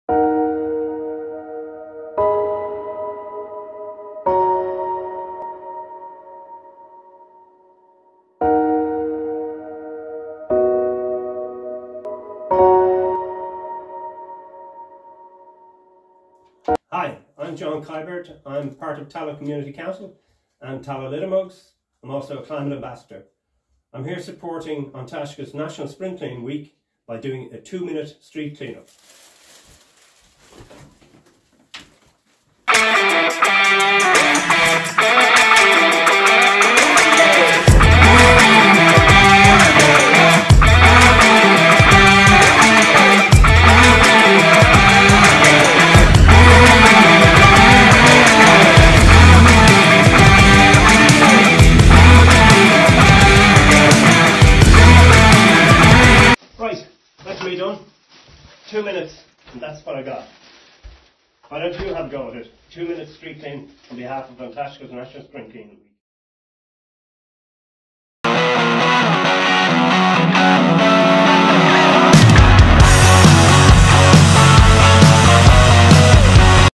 2021 Spring Clean Tallaght, John Kiberd.
Part of the Tallaght community Spring Clean campaign 2021.
John Kiberd.
Tallaght Littermugs.